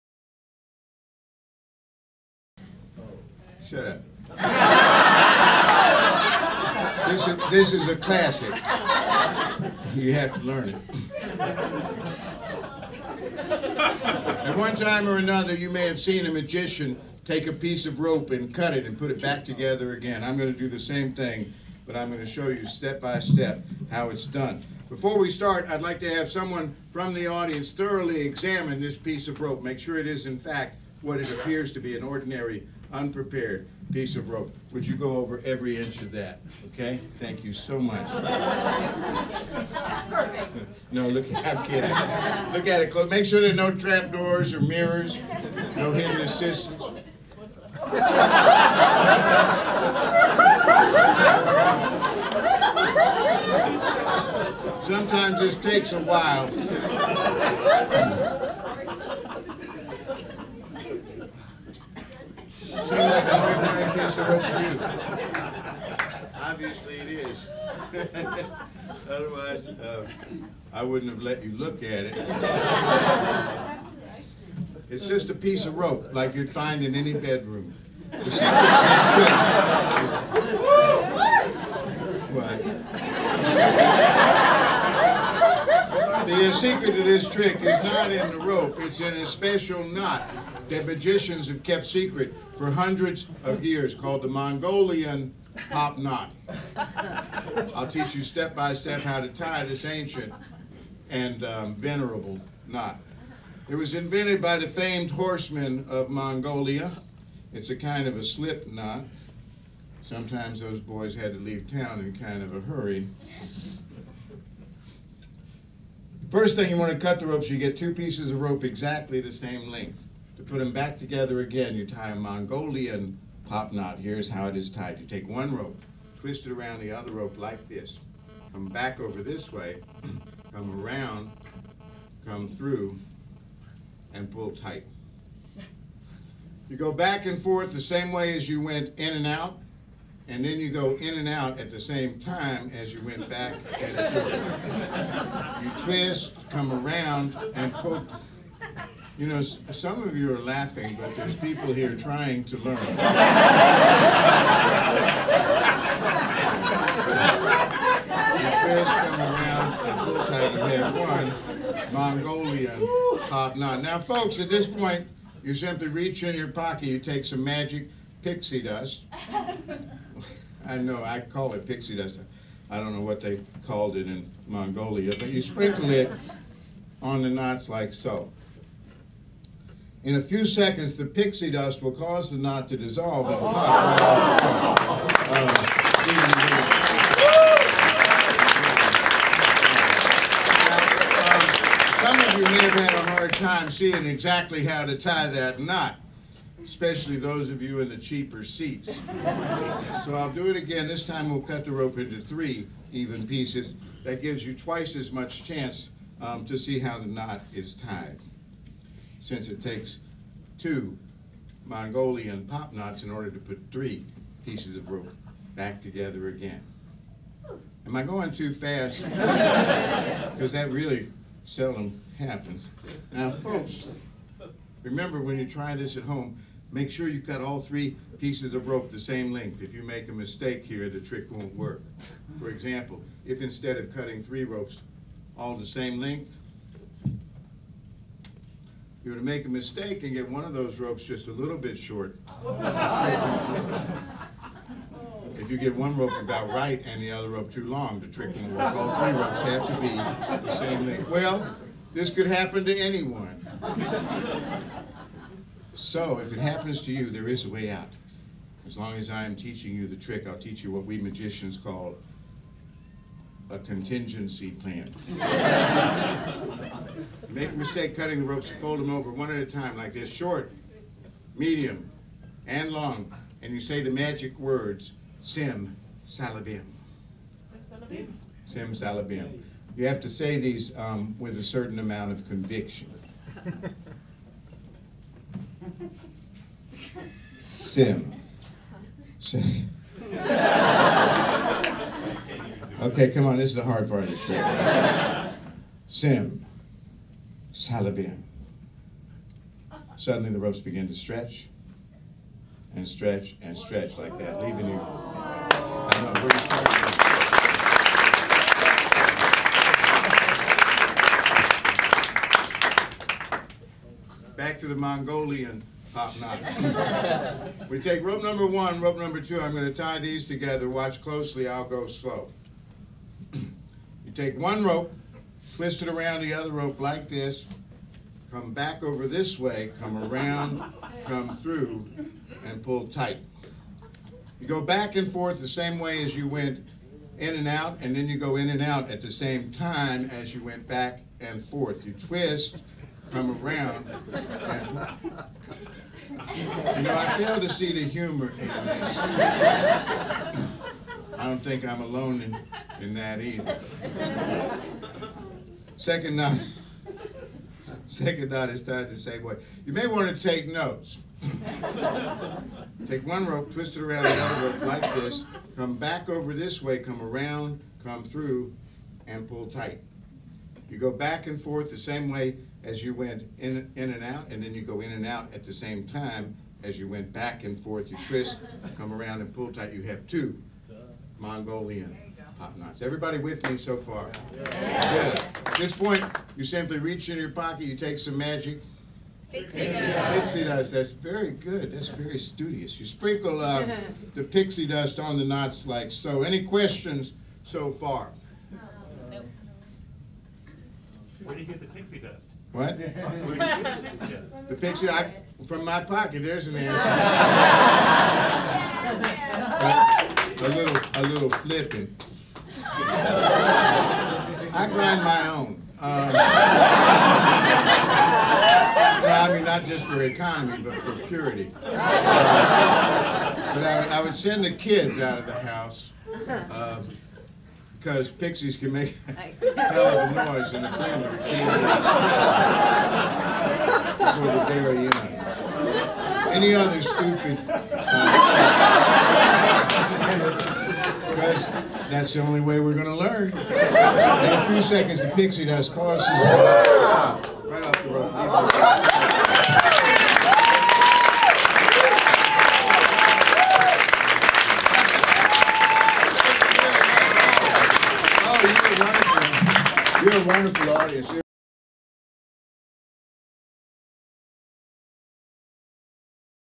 Mongolian Pop Knot Oct 2016
Pop Haydn performs his Mongolian Pop-Knot routine in the Palace of Mystery at the world-famous Magic Castle in Hollywood, California.

October, 2016.